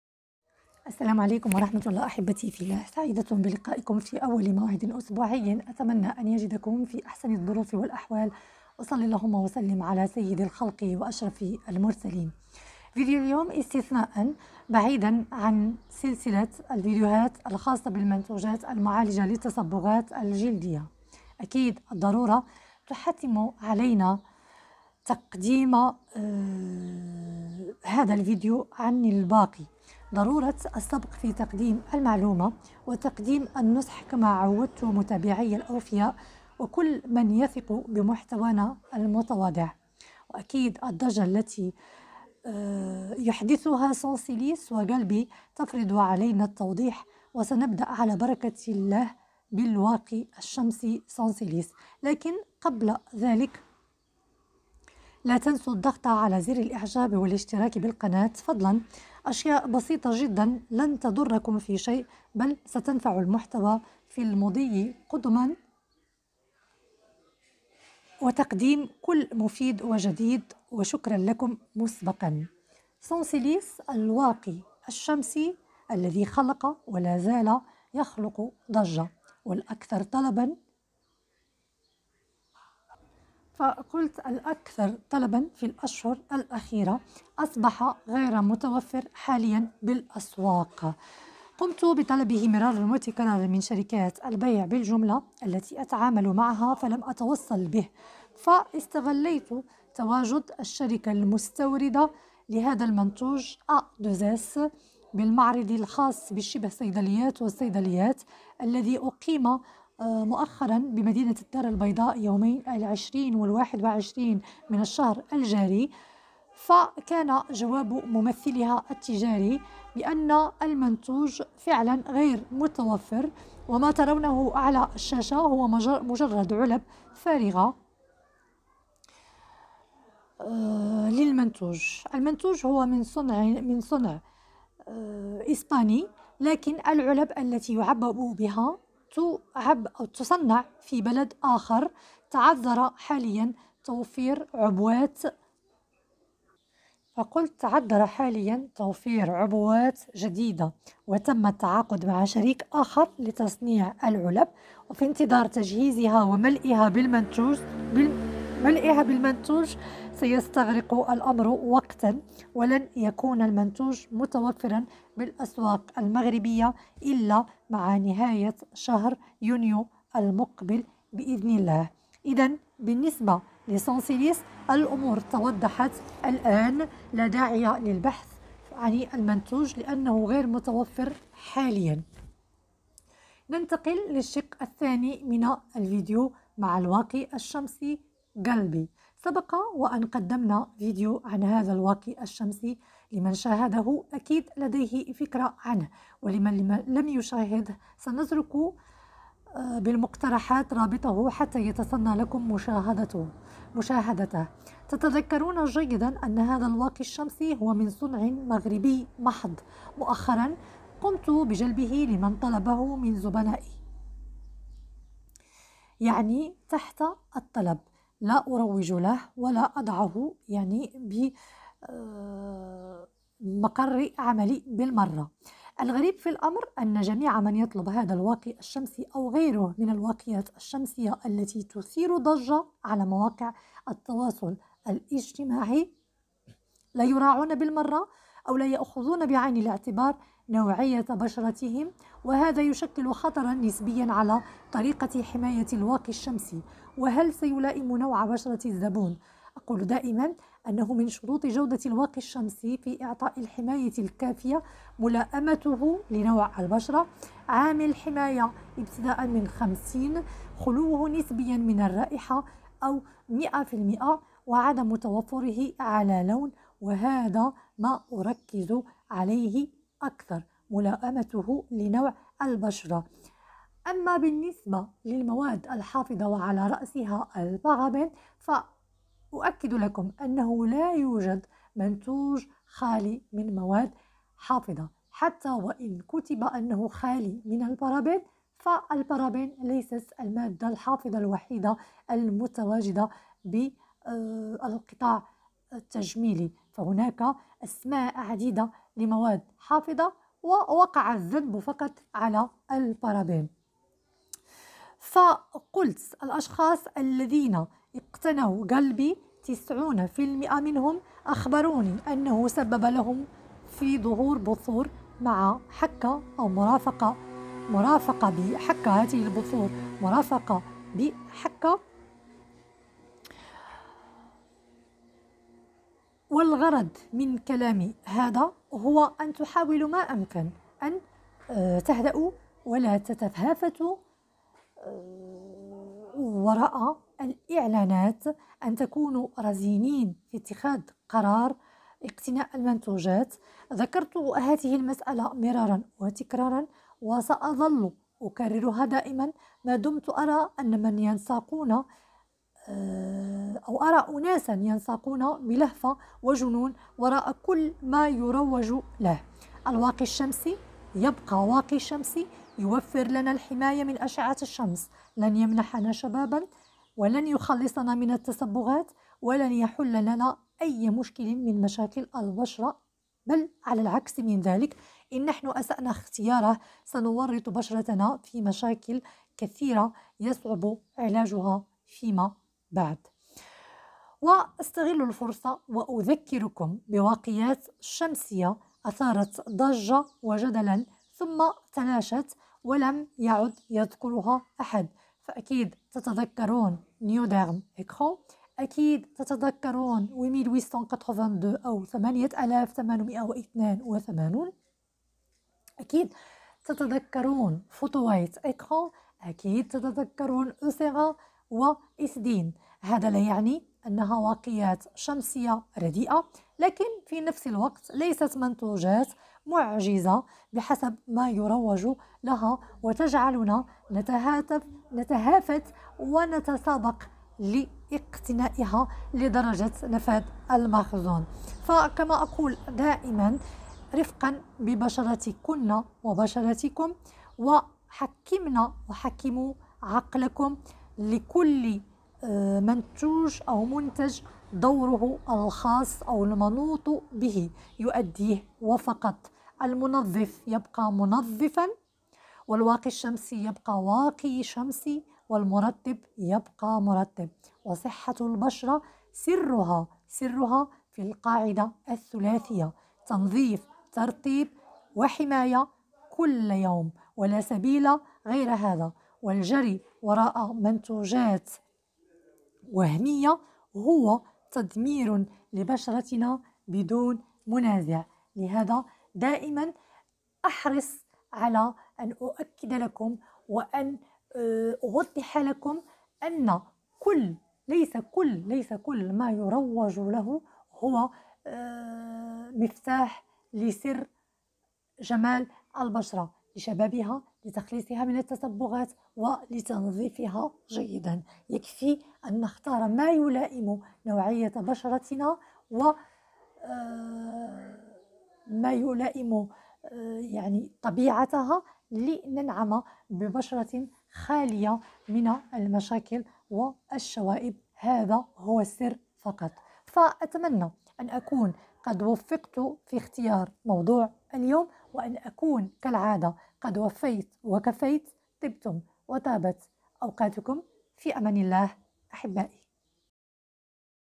Sensilis/Galby واقيات شمسية تثير حركة تجارية مهولة :كيف نميز الجيد من الرديء؟
ضع واقي الشّمس بانتظام يحد من إصابة البشرة بعلامات تقدُّم السن والشيخوخة المبكرة، ووفق ما أوردهُ بعض الباحثيين في مجلة Annals of Internal Medicine في يونيو/حزيران عام 2013م بعد إجراء دراسة تمّت فيها المقارنة بكمية واقي الشمس المُوصّى باستخدامها مقابل الكمية التي يُقدرها أشخاص من تلقاء أنفسهم تقل أعمارهم عن الخمسة وخمسين عاماً، وبعد مُضي أربع سنوات ونصف على تطبيق الدراسة وجد الباحثون أنّ الفئة المستخدمة لواقي الشمس بانتظام وفق الكمية المُوصى بها كانوا أقلّ عرضة للشيخوخة المبكرة بنسبة 24% مقارنةً مع الفئة الأخرى والتي كانت تستخدم واقي الشمس حسب تقديرها الشخصي، وقد تمّ قياس الشيخوخة بتقدير نسبة الخطوط الرفيعة وملمس الجلد الخشن، كما أشاد المعهد الوطني للشيخوخة بأنّ تطبيق واقي الشمس على البشرة يمنع ظهور البقع والتّصبغات التي تظهر مع تقدّم العمر.